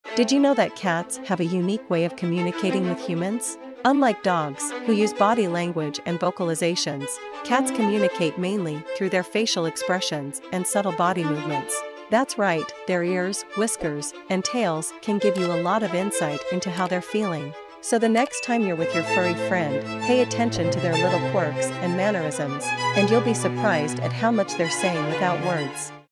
The Secret Language of Cats: What You Need to Know!
In the video, the creator starts off by posing the question, "Did you know that cats have a secret language that they use to communicate with humans?" They then proceed to share fascinating facts about how cats use their ears, tails, and body language to convey their moods and intentions. The creator provides specific examples of different ear positions and tail movements and what they might indicate about a cat's mood. The video is fast-paced and includes eye-catching visuals of cats demonstrating different body language cues. The creator ends the video by encouraging viewers to pay closer attention to their cats' body language to better understand their furry friends.